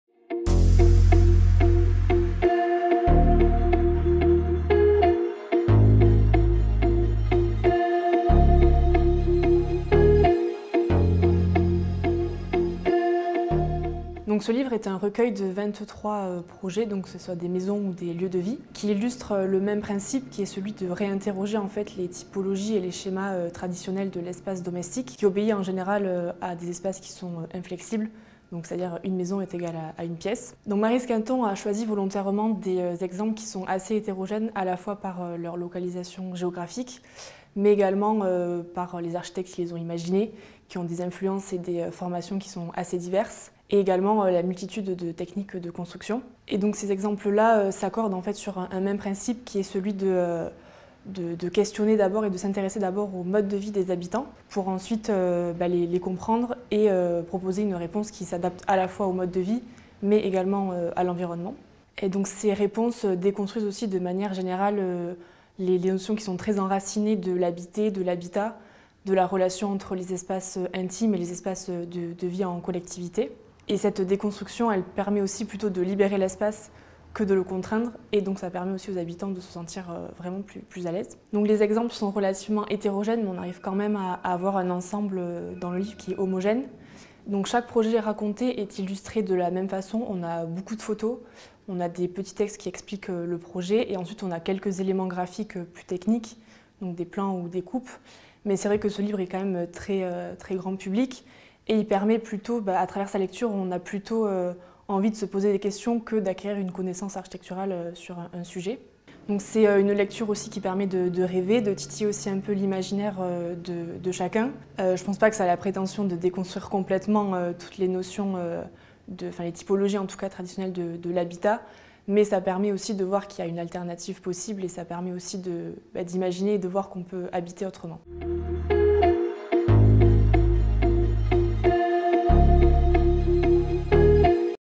Archi'Lecture - Habiter autrement
Laura Vidal, étudiante à l'Ecole d'architecture et de paysage de Bordeaux vous présente "Habiter autrement : quand l'architecture libère la maison" de Maryse Quinton aux éditions La Martinière.